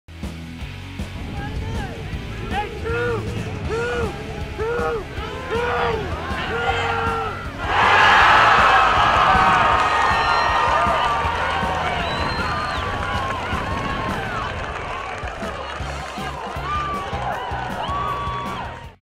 Cornell Pulls off Hidden Ball Trick Against Hobart | Lax.com Quick Clips
- In their 12-8 win over Hobart to open the 2014 season, Cornell midfielders John Hogan and Joe Paoletta pulled off the old "hidden ball" trick. Check it out and see the full game highlights